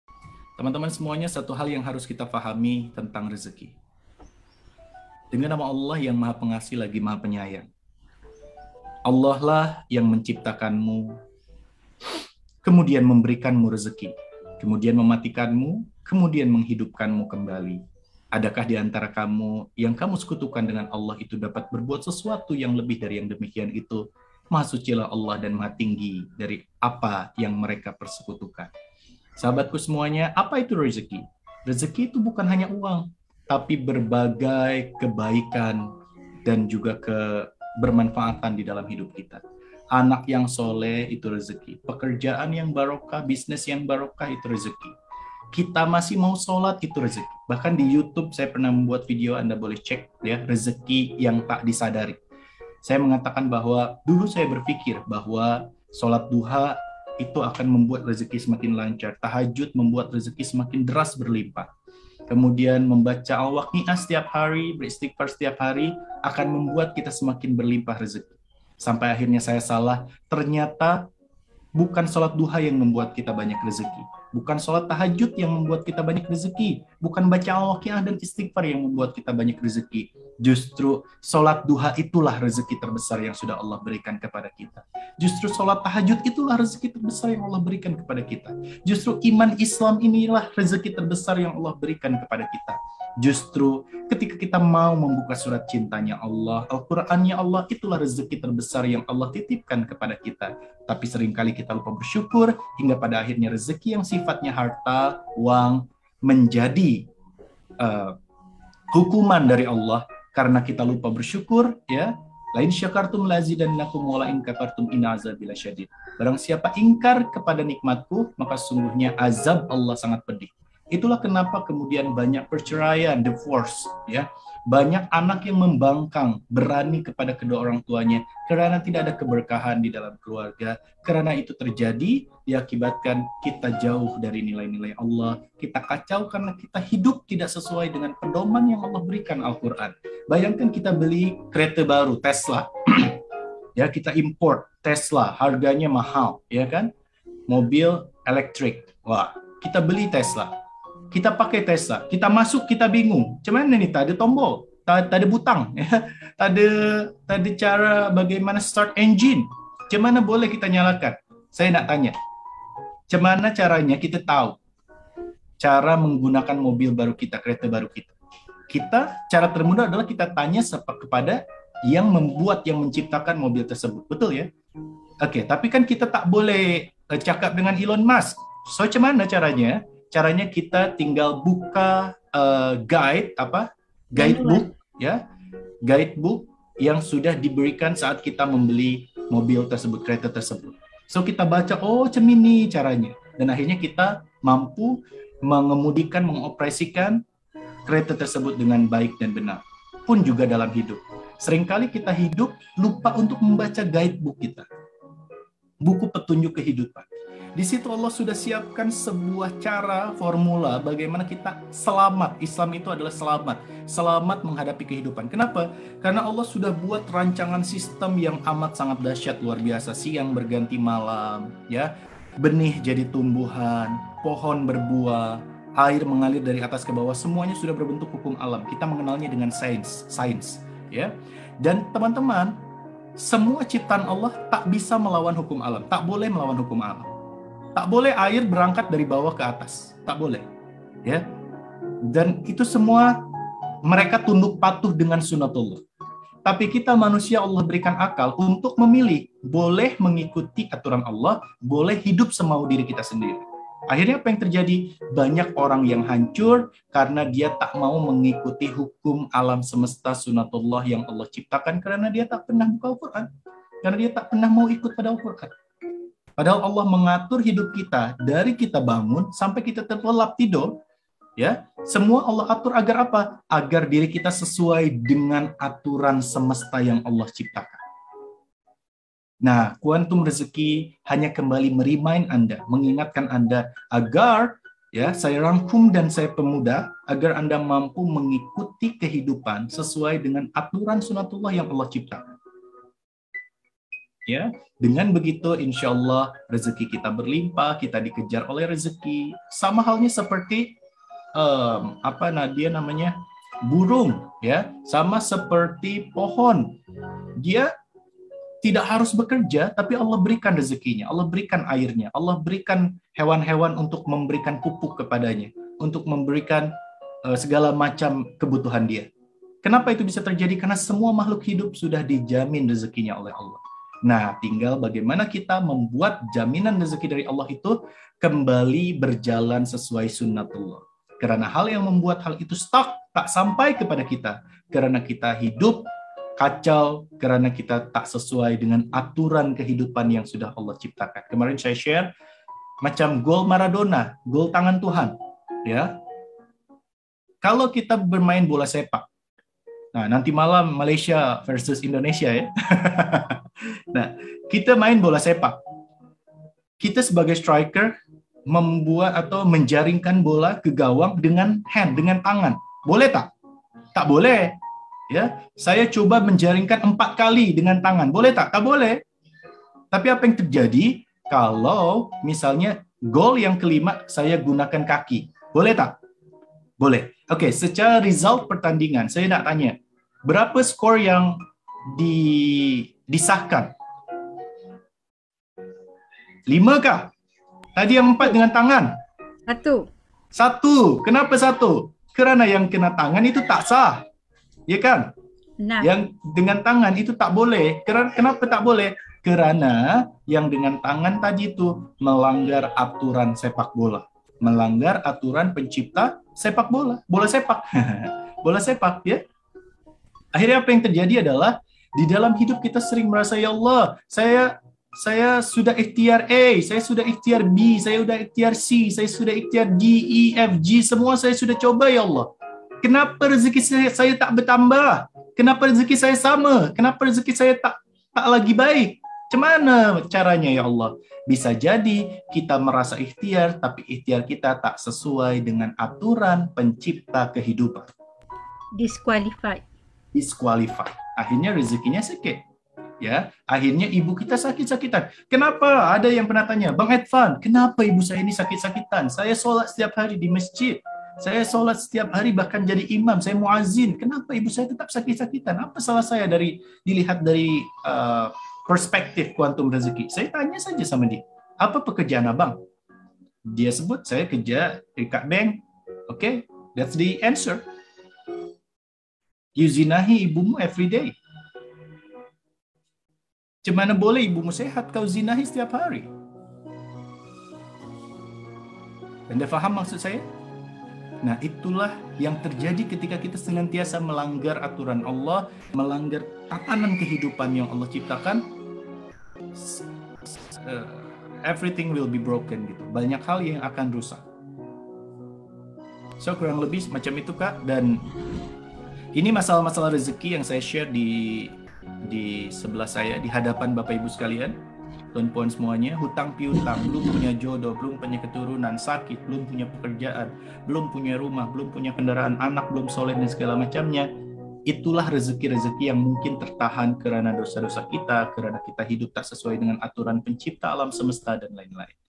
SOLUSI DARI PERMASALAHAN REZEKI | Quantum Rezeki Malaysia
SOLUSI PERMASALAHAN REZEKI | Preview Quantum Rezeki Malaysia

Jom Join Quantum Rezeki di Selangor Malaysia.

Untuk Sahabat-sahabat Saya di Malaysia, In Shaa Allah pada tarikh 6 Ogos 2022, Saya akan bina sebuah majelis pelatihan Quantum Rezeki di Kajang, Selangor, Malaysia.

Sila ajak keluarga, saudara, sahabat dan siapapun yang teruja untuk mendapatkan lonjakan Rezeki dalam hidup ya.

Sampai Jumpa!